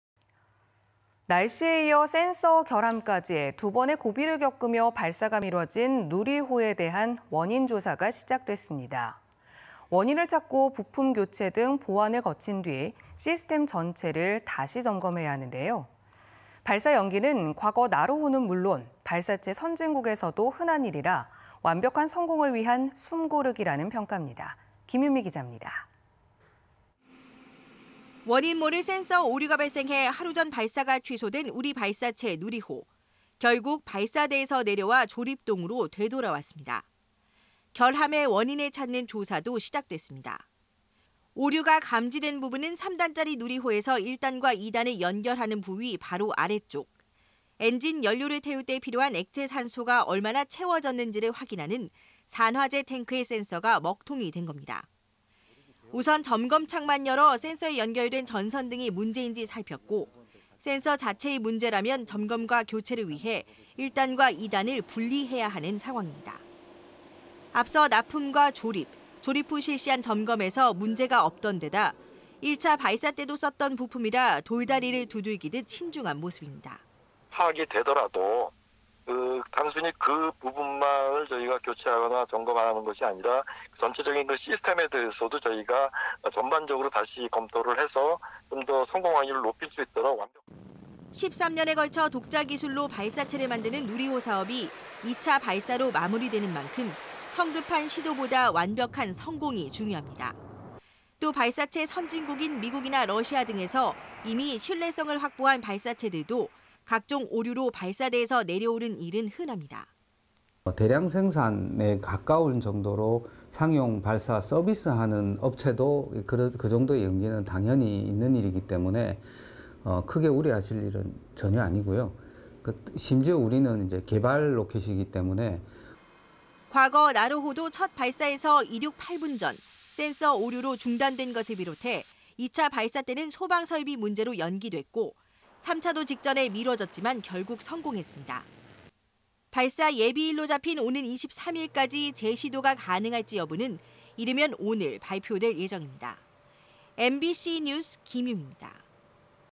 누리호 결함 원인 조사 착수 '신중 또 신중'/대전MBC
"날씨에 이어 센서 결함까지, 두 번의 고비를 겪으며 발사가 미뤄진 누리호에 대한 원인 조사가 시작됐습니다. 발사 연기는 과거 나로호는 물론, 발사체 선진국에서도 흔한 일이라 완벽한 성공을 위한 숨 고르기라는 평가입니다"